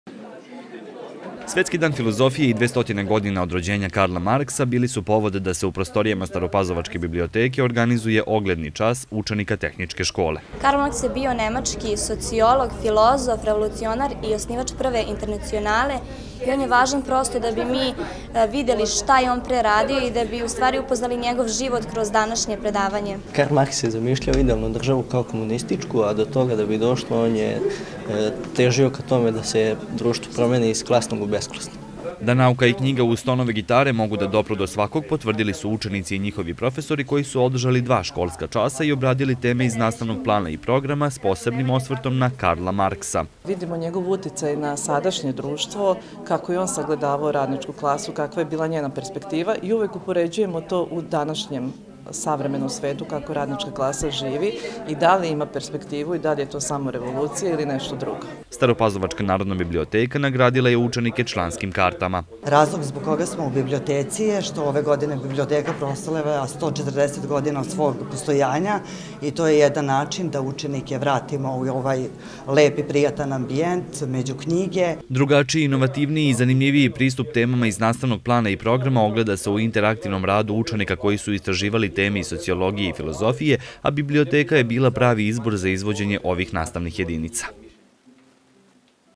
Ogledni čas učenika Tehničke škole u staropazovačkoj biblioteci
Da se teme iz sociologije i filozofije iz nastavnog programa mogu prikazati na drugačiji, inovatnivniji i zanimljiviji način pokazali su učenici Tehničke škole u Staroj Pazovi koji u saradnji sa profesorima i Narodnom bibliotekom “Dositej Obradović” organizovali ogledni čas.